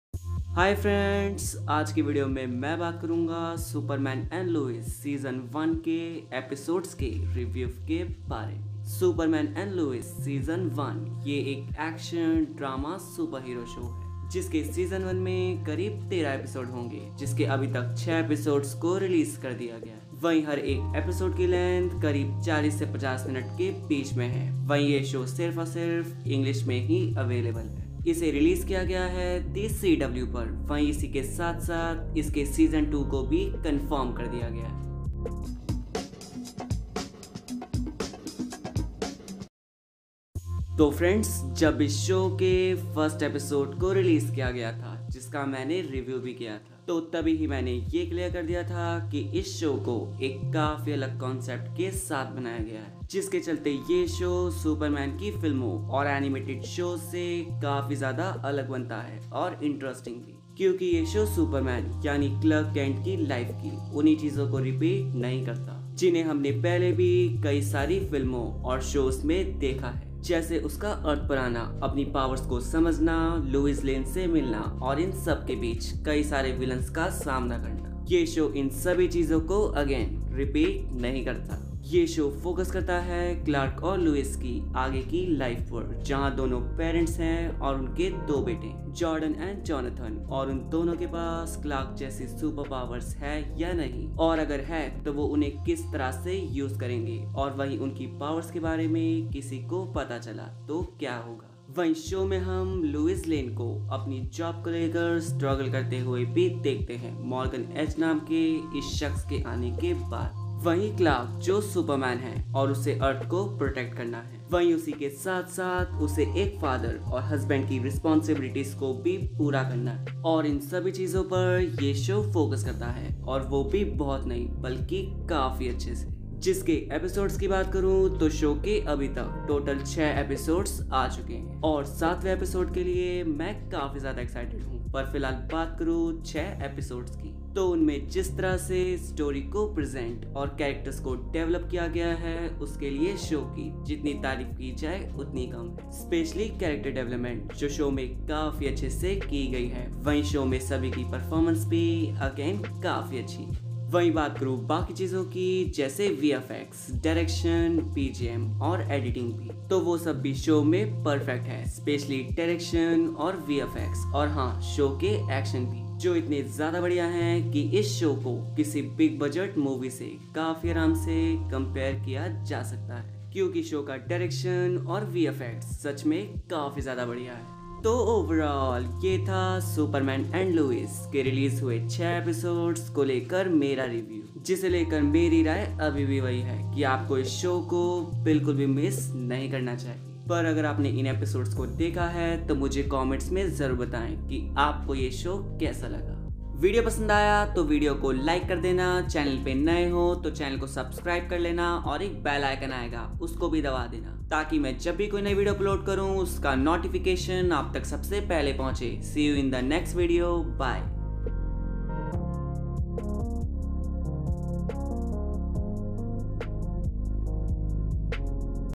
SUPERMAN AND LOIS : Season 1 Review | EP1 - EP6 Review | IN HINDI
SUPERMAN AND LOIS : Season 1 Review | EP1 - EP6 Review | IN HINDI | Superman And Lois Review

Hi Friends Aaj Ke Video Me Hum Baat kare Ge Superman And Lois Season 1 Ke Episode 1 - 6 Tak Ke Review Ke Baare Me Video ko End Tak Zaroor Dekhna Pasand Aaya Toh Like Comment Apne friends Ke Saath Share Zaroor Karna.